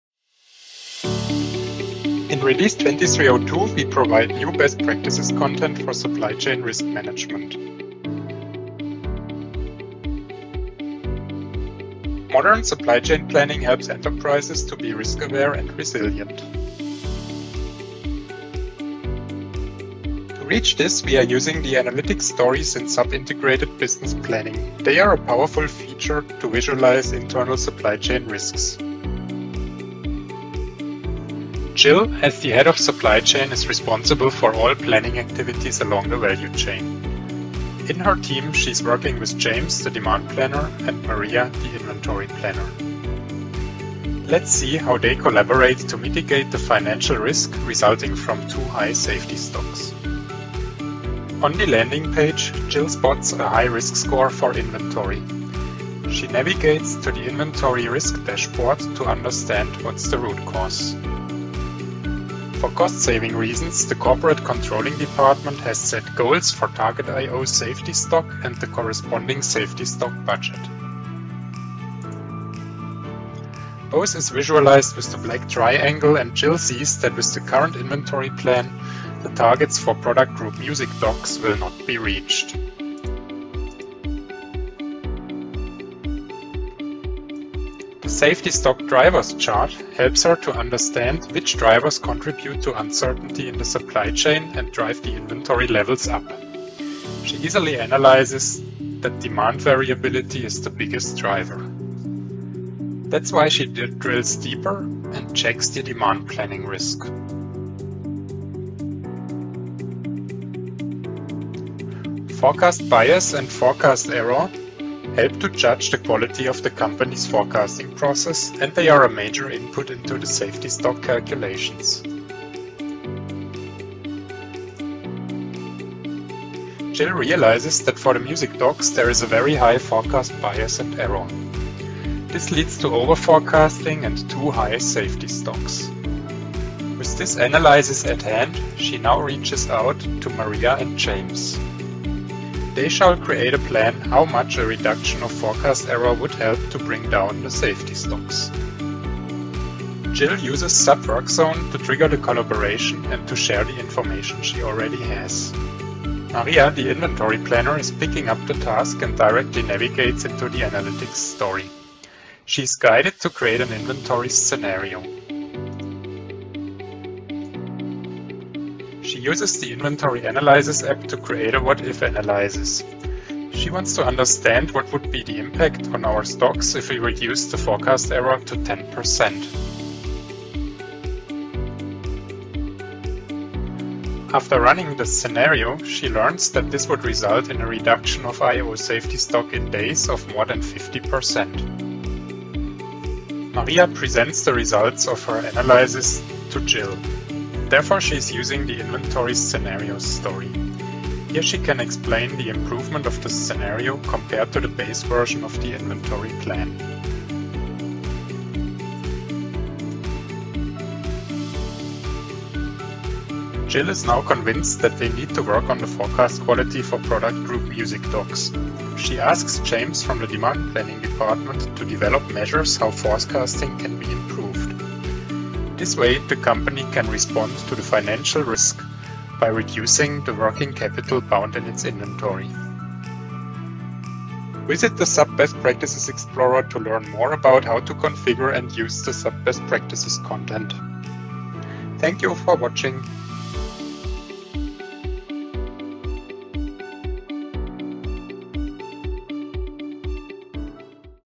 SAP IBP 2302 Release Highlight: Supply Chain Risk Management
Find out how to use SAP Integrated Business Planning to analyze and respond to supply chain risks.